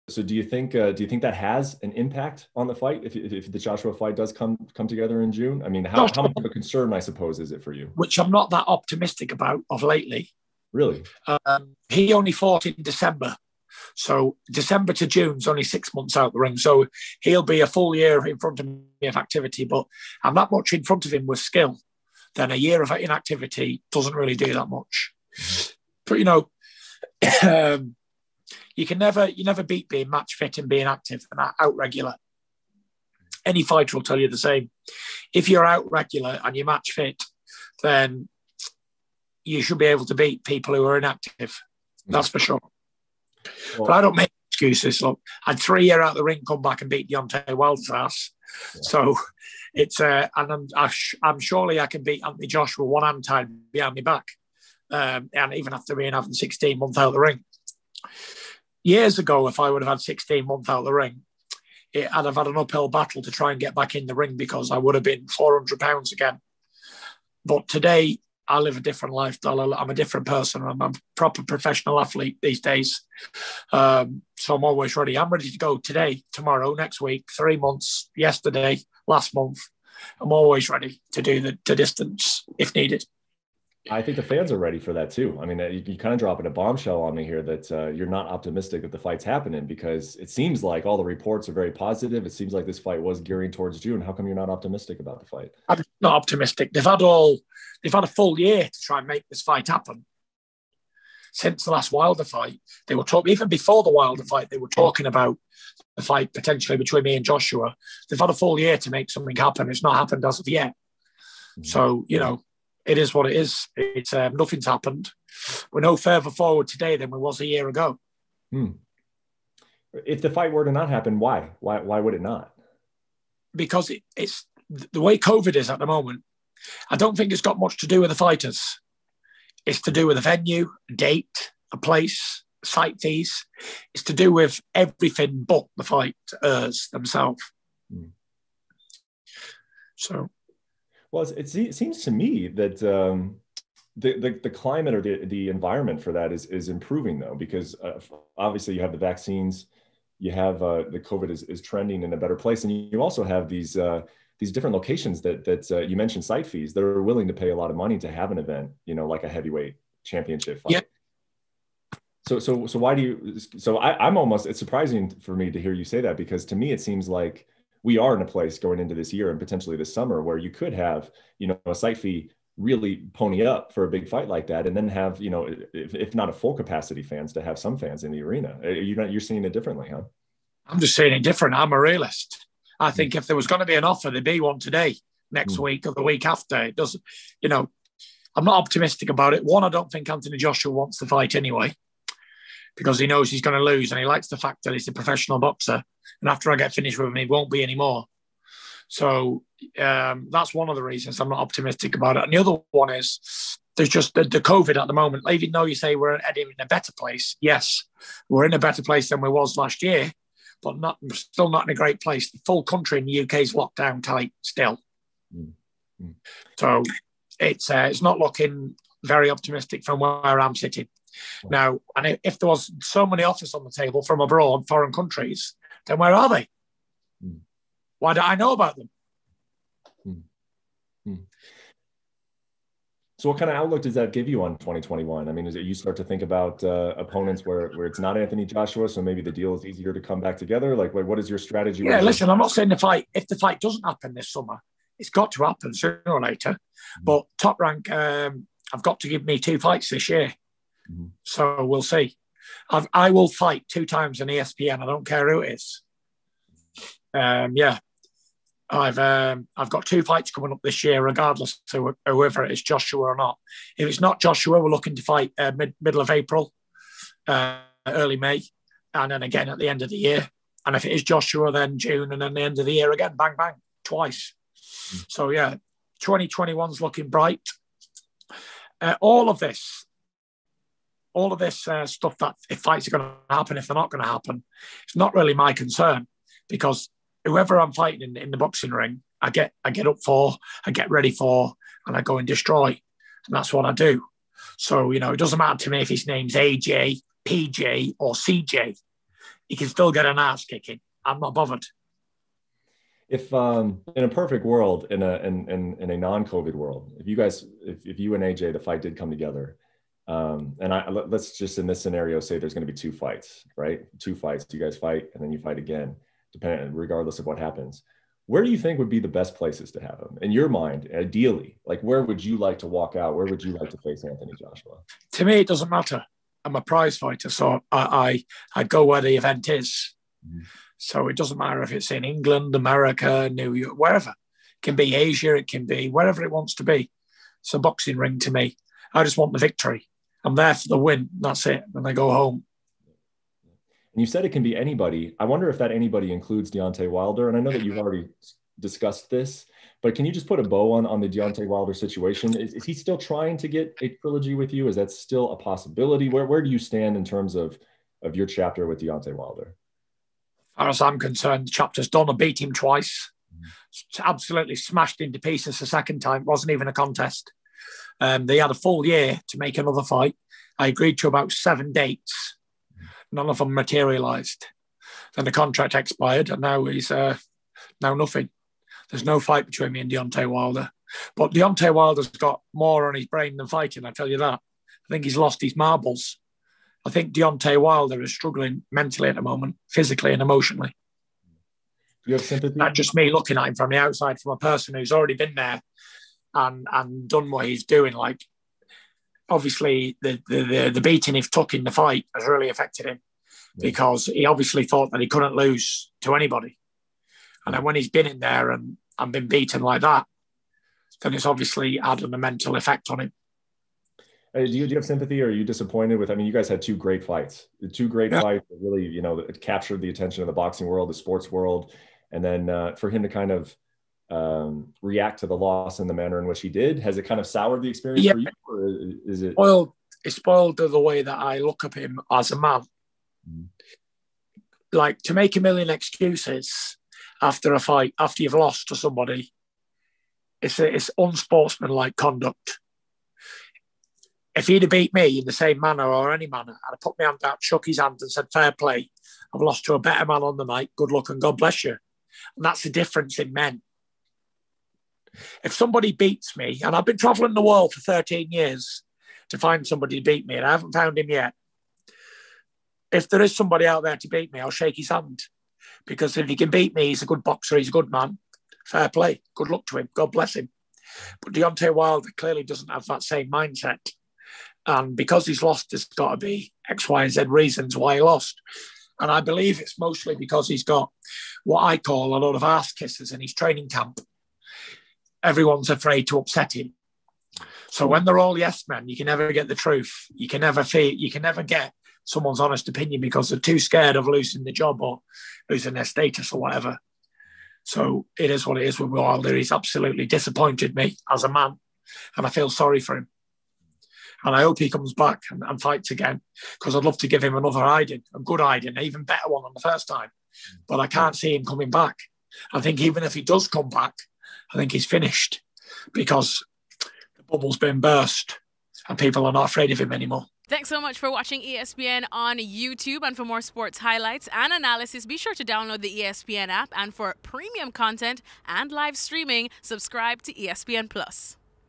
Tyson Fury DOESN'T think that fight with Anthony Joshua will happen in June | ESPN Boxing
ESPN's Brett Okamoto talk's exclusively with world heavyweight champion TYSON FURY about his potential upcoming fight with fellow British heavyweight Anthony Joshua. Fury believes a fight in June is unlikely and that Joshua doesn't want to fight him in fear of losing badly to the 'gypsy king'. Fury explains how he will be fighting twice this year on ESPN, hoping for fights in April/May and December. A trilogy with Deontay Wilder is also unlikely according to Fury as the rematch clause has expired in the contract between the fighters, Fury also believes Wilder is battling outside the ring and therefore doesn't see a fight with him any time soon.